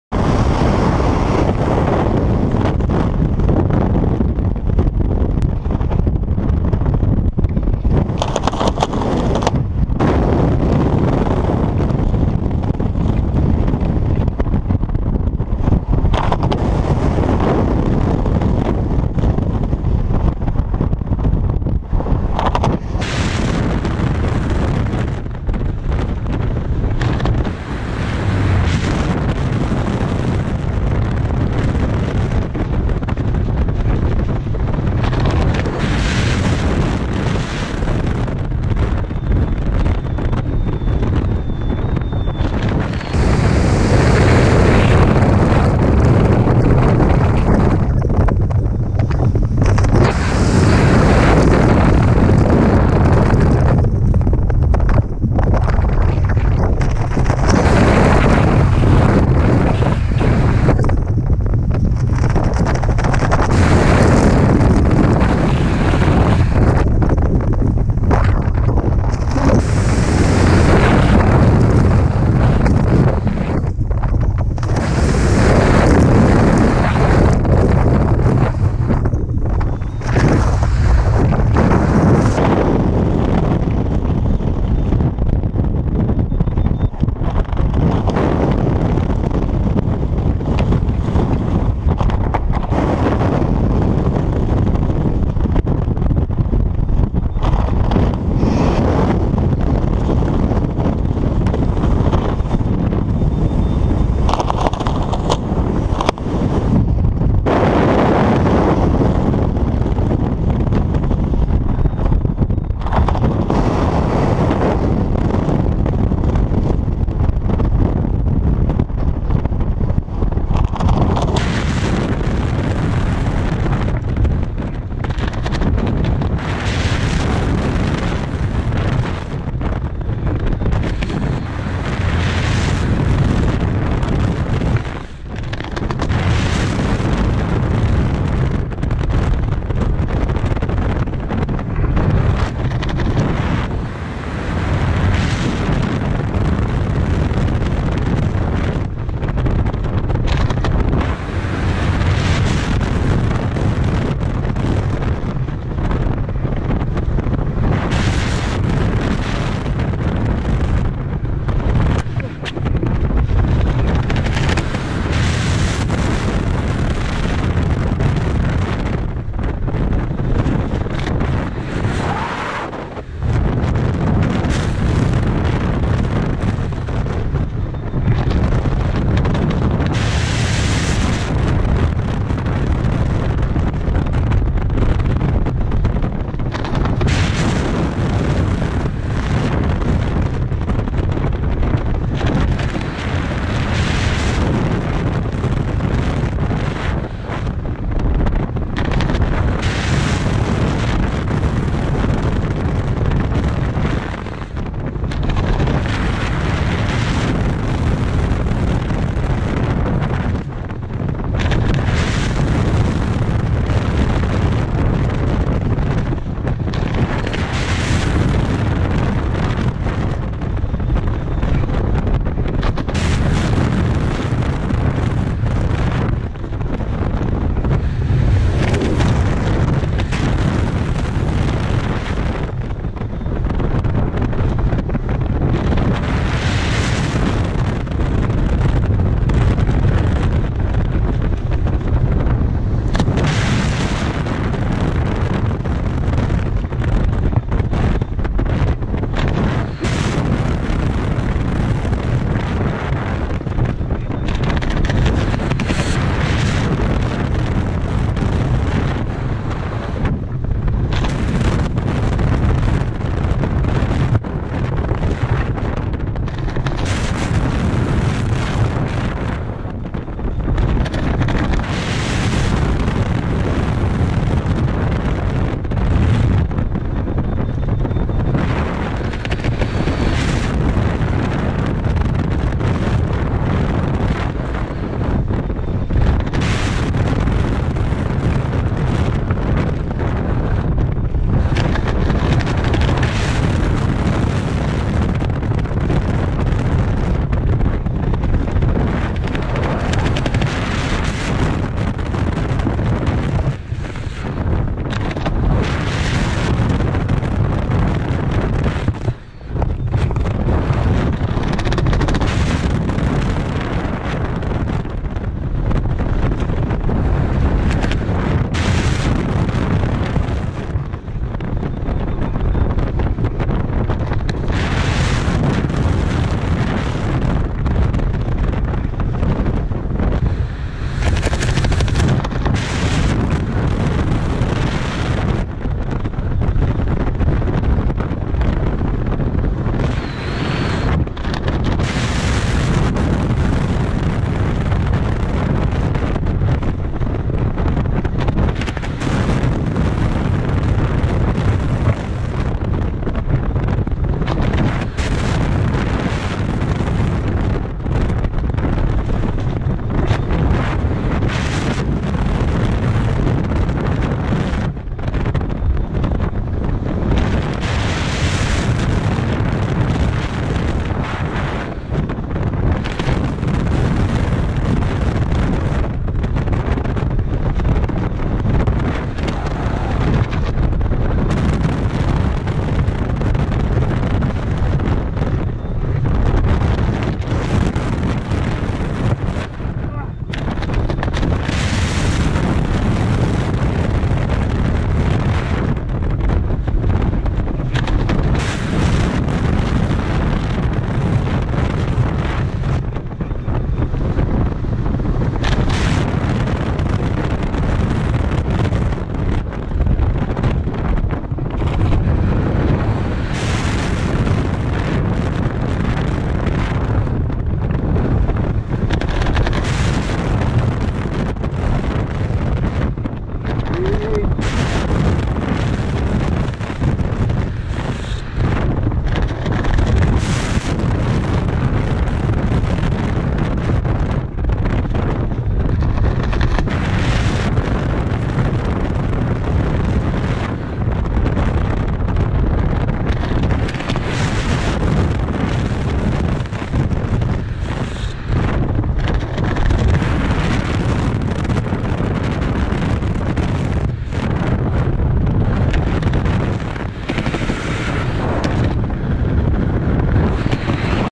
Velocity 103 Openings
A compilation of 70 openings of my Velocity 103 sqft, that I used between 2017/2018.

Wingload: 2,0
Average experience: 800 / 1000 jumps